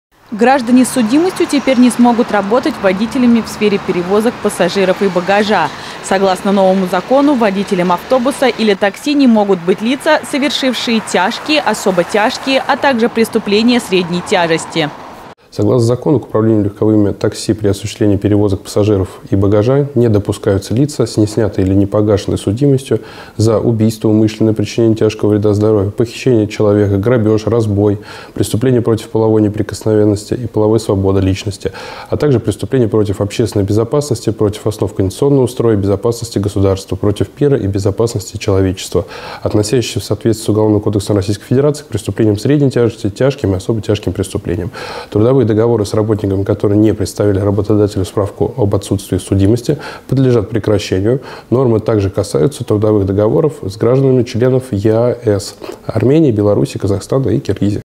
Прокуратура разъясняет 7: Перевозка пассажиров и багажа
Гражданам с судимостью отныне закрыта дорога в сферу перевозок пассажиров и багажа. Теперь – согласно новому закону – водителем общественного транспорта или такси не могут быть лица, которые совершили тяжкие, особо тяжкие, а также преступления средней тяжести. Подробнее об этом рассказали сотрудники Одинцовской городской прокуратуры.